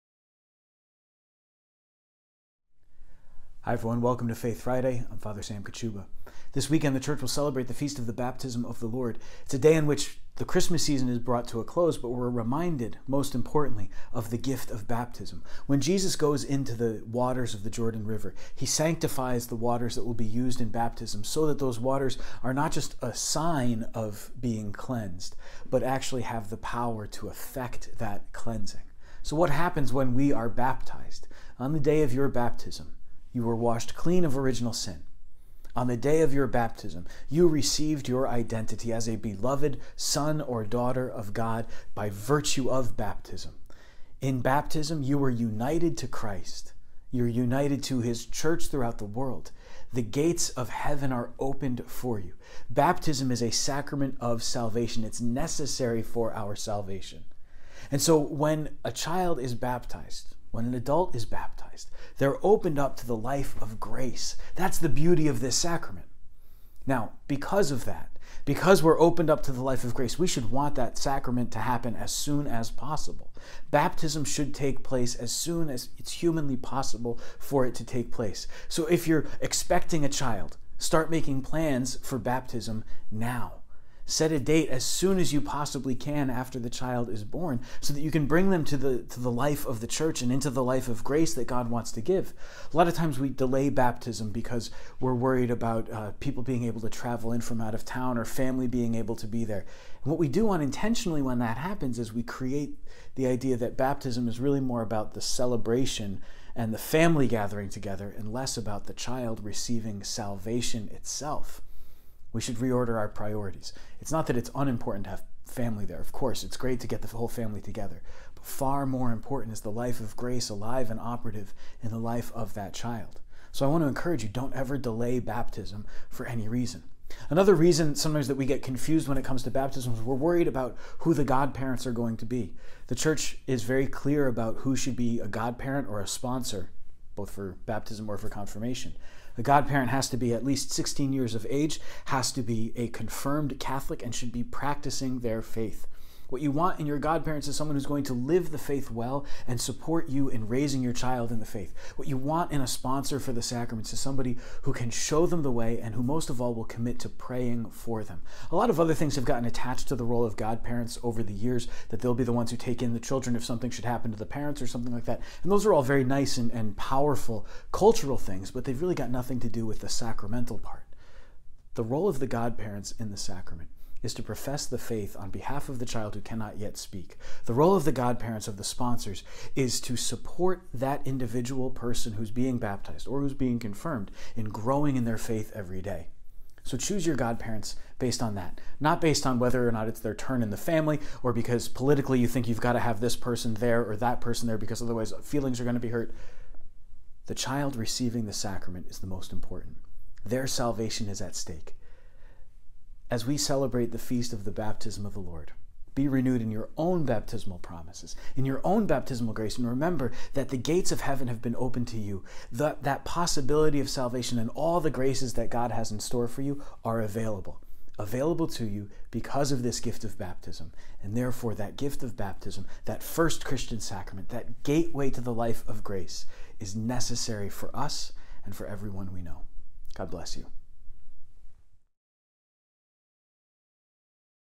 Baptism | Faith Friday
This weekend, the Church celebrates the Baptism of the Lord, which officially closes the Christmas Season. Fr. Sam reflects on our own Baptisms, and what this beautiful sacrament means for us!